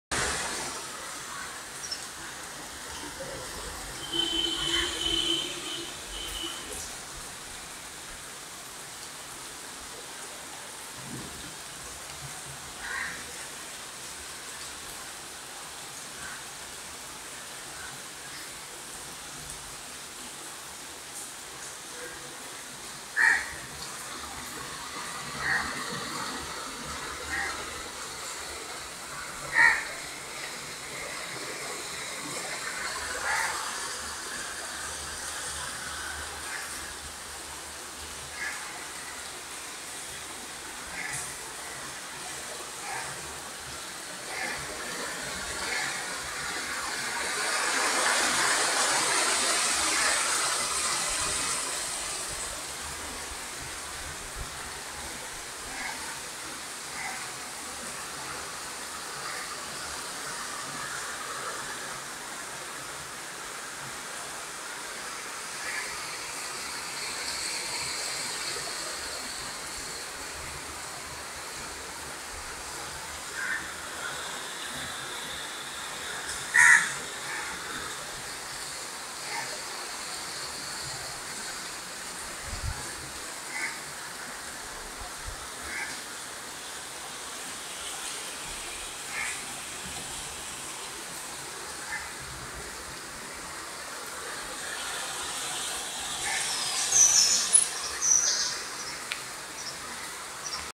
ASUS Zenfone 5 A501CG Full HD Video Sample
Asus Zenfone A501CG Full HD sample shot during the rainy season. All settings are default and there is no Video Stabilization Enabled.